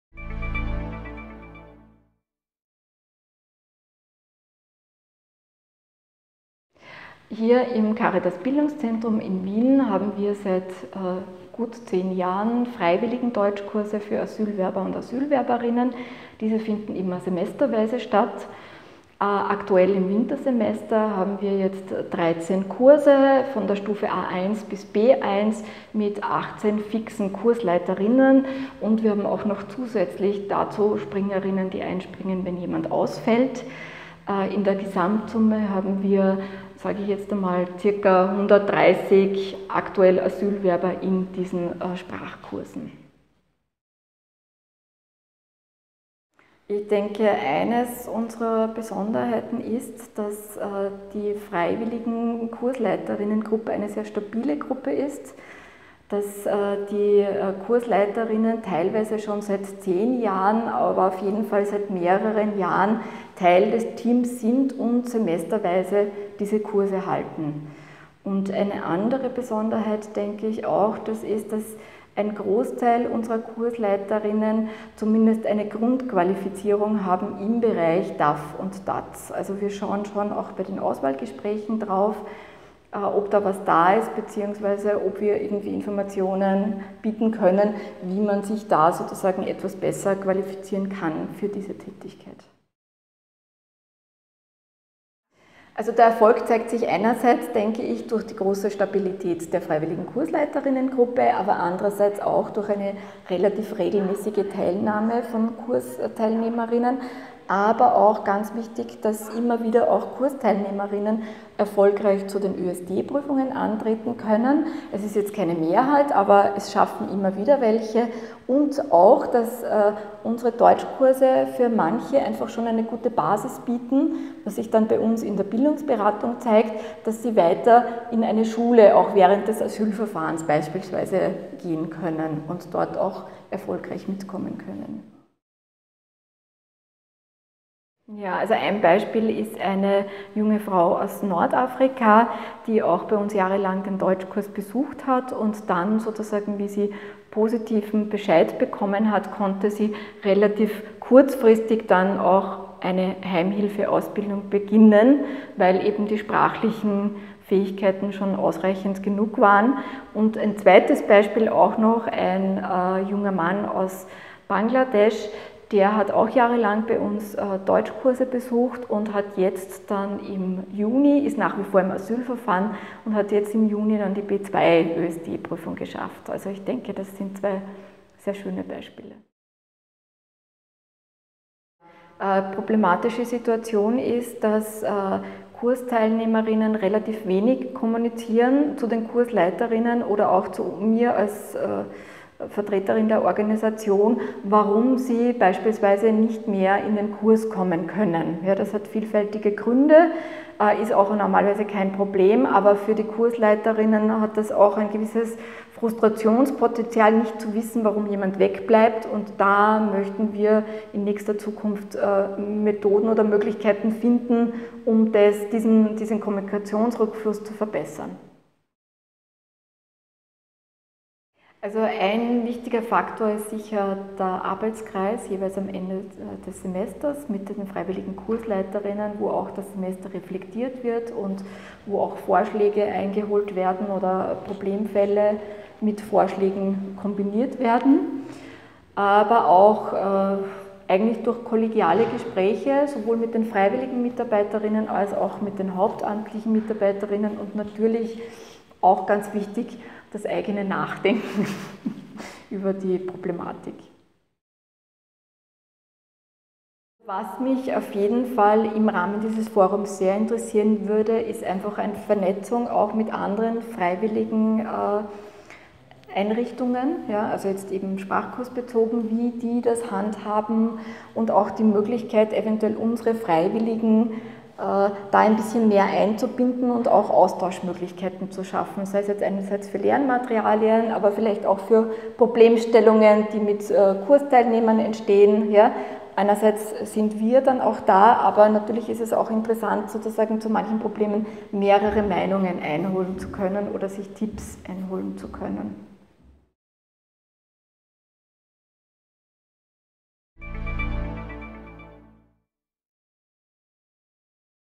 Caritas Bildungszentrum setzt auf Stabilität der Kursleitendengruppe
In diesem Video gibt Gudrun Schlosser, Koordinatorin der Freiwilligendeutschkurse für Asylwerberinnen und Asylwerber im Caritas Bildungszentrum Wien, Auskunft darüber, welchen Ansatz ihre Einrichtung bei der Organisation und Unterstützung der Freiwilligengruppe verfolgt, worin sie die besondere Qualität der Einrichtung erkennt und welches Entwicklungsvorhaben sie aktuell verfolgt. Sie ist offen für Erfahrungsaustausch mit anderen Einrichtungen.